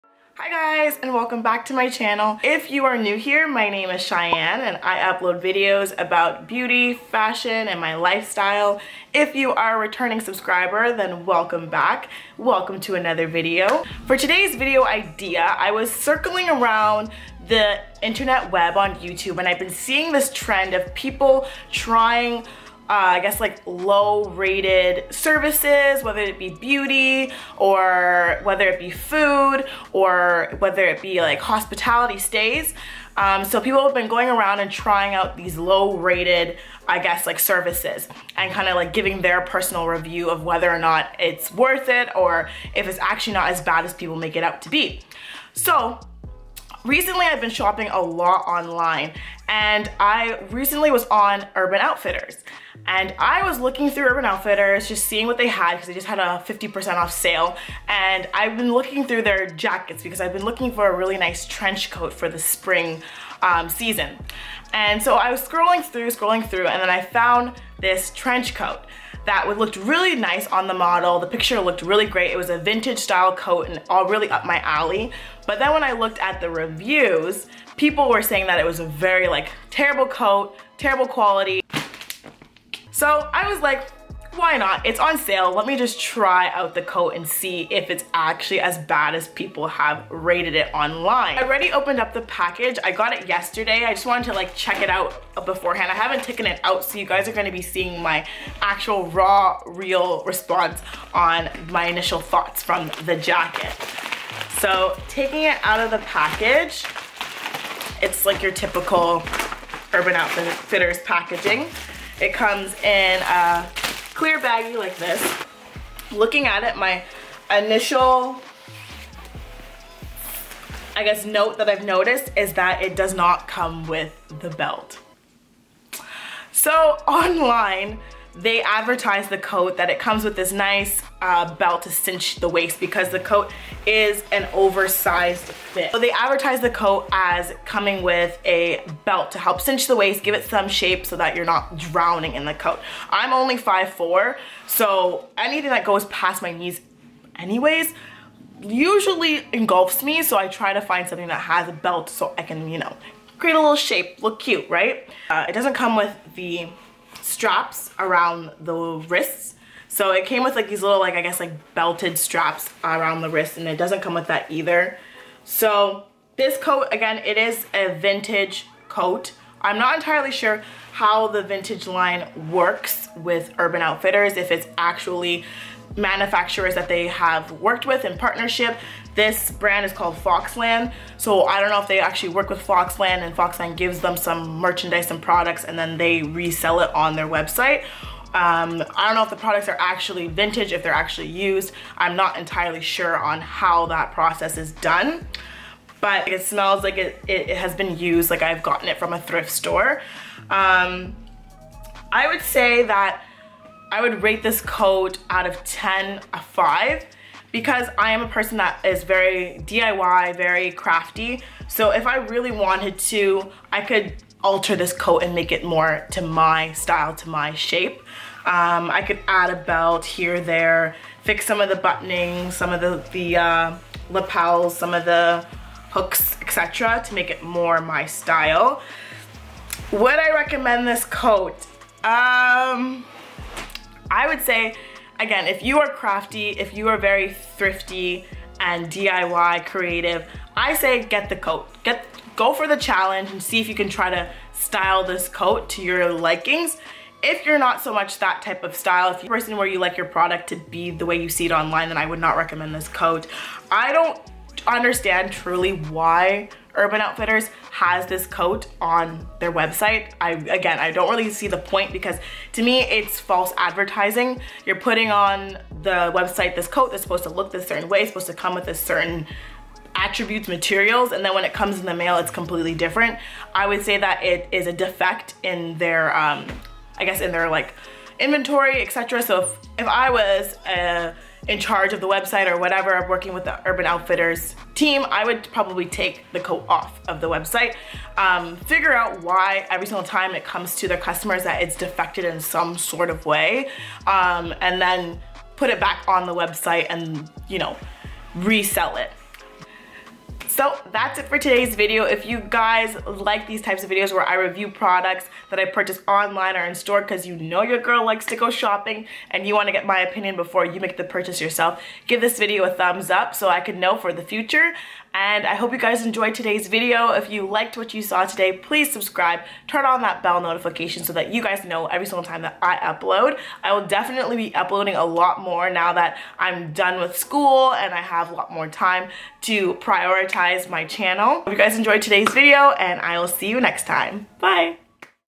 SCAMMED...Buying the WORST Reviewed Coat Online! | Shiane Johnson
Hey guys, watch me review the lowest reviewed coat from OU! DISCLAIMER this videos just for fun and educational, I still enjoy shopping at OU!